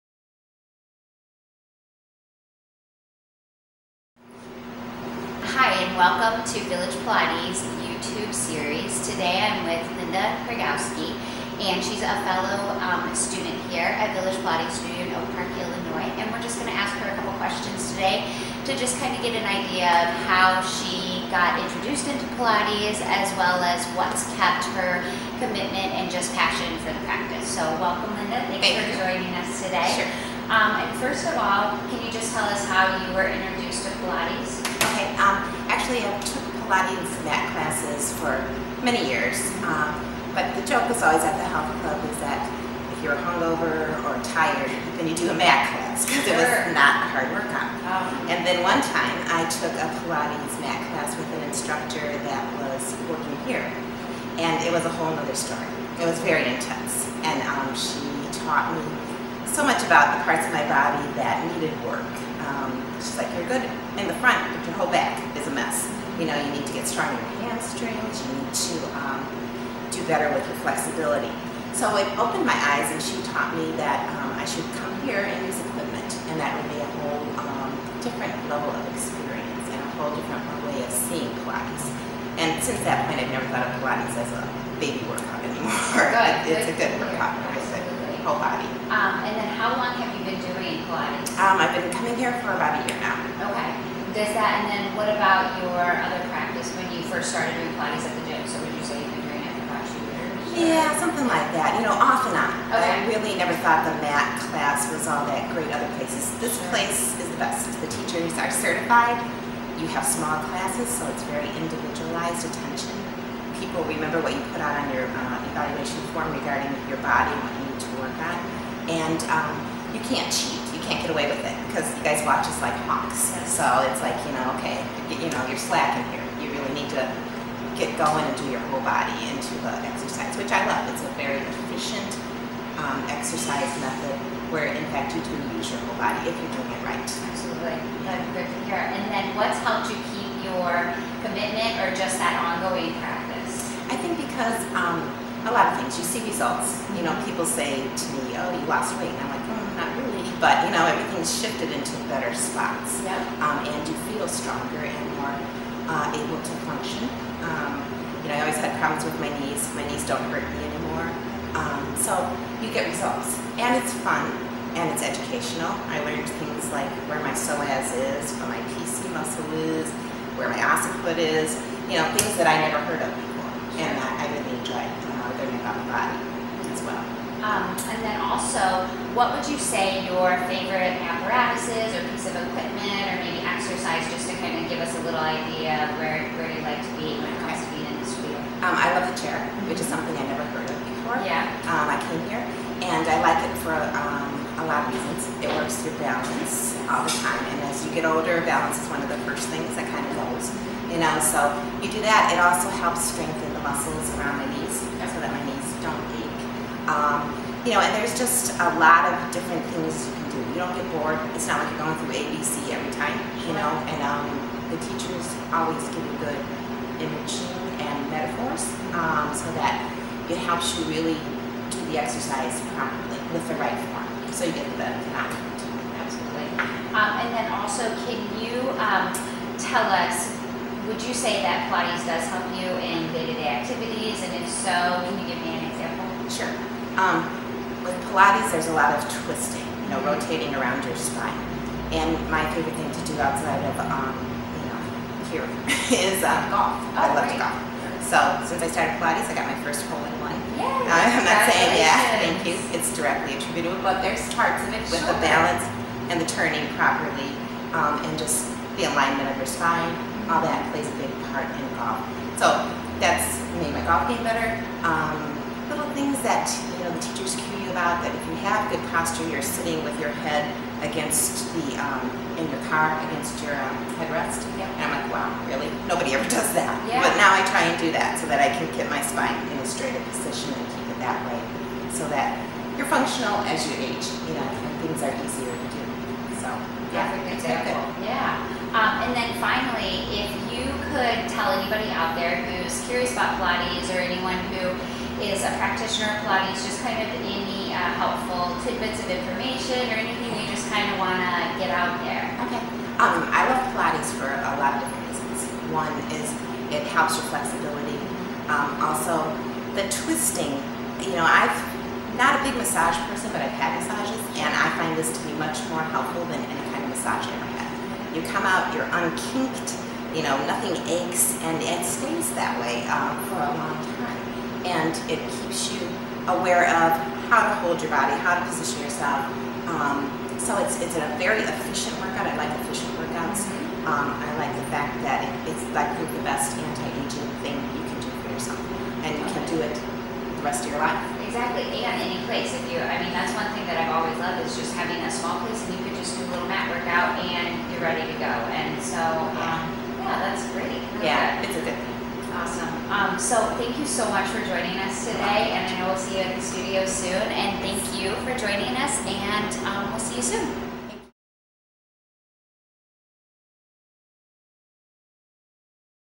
Linda Testimonial
Angie Dalrymple, interviews a long time client and Pilates enthusiast, Linda Kyrgowski.